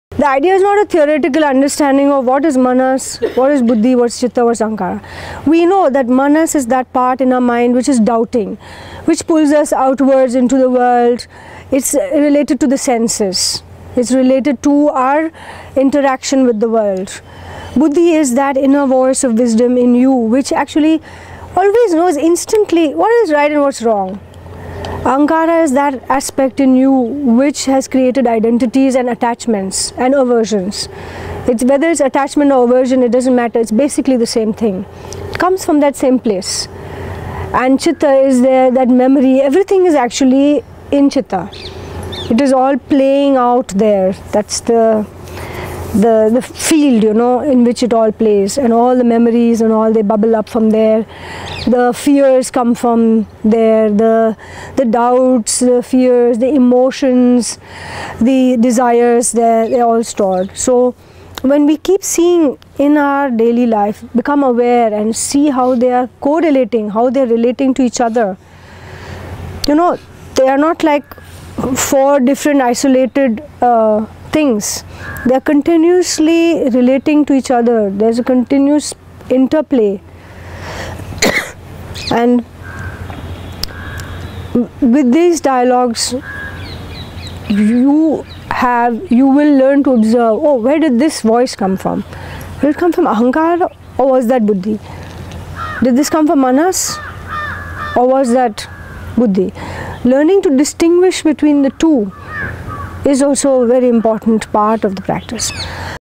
Yoga Meditation and 4 aspects of the mind
Radhikaji, teacher of Samaya Srividya describes the 4 aspects of the mind. This was recorded in a retreat in Rishikesh, India. For more information or to join our online meetings